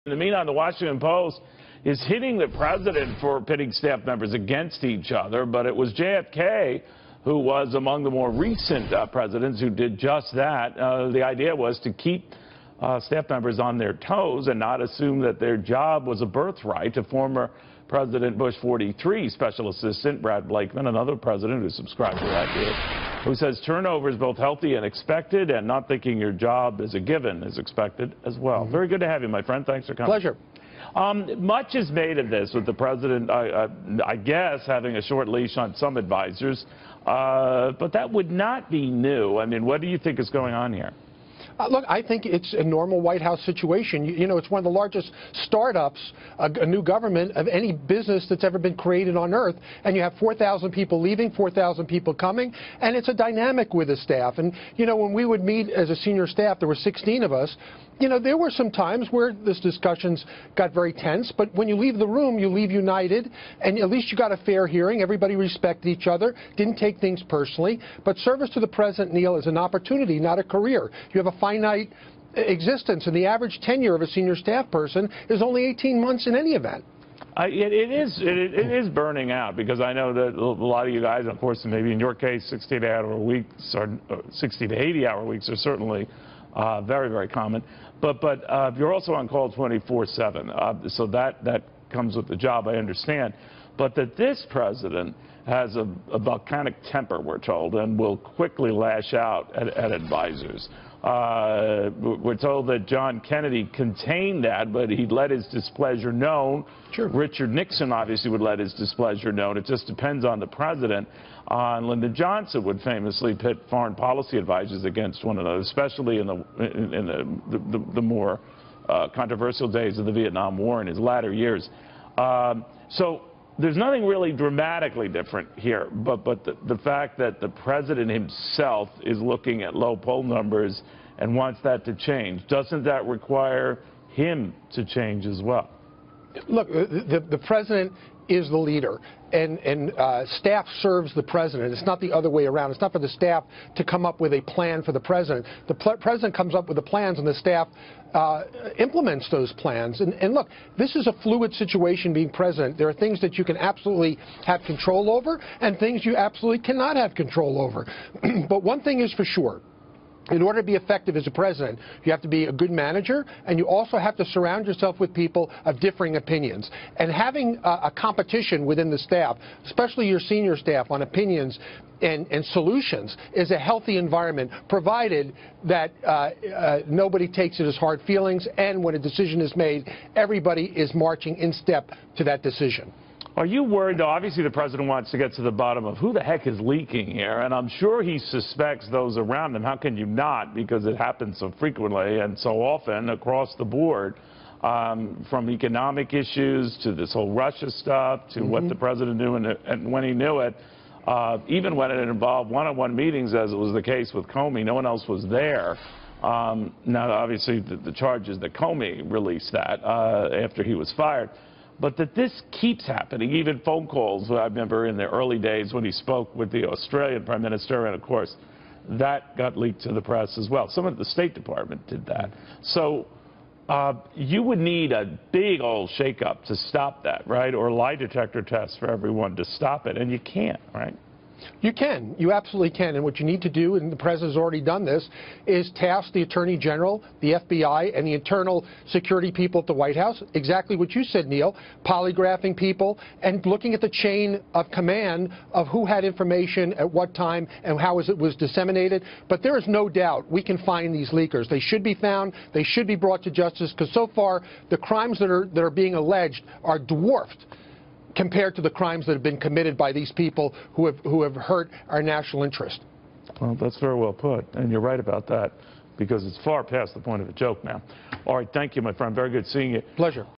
An inside look at the White House staff dynamics
Former special assistant to President George . Bush, Brad Blakeman, on the behind the scenes dynamics within the White House staff.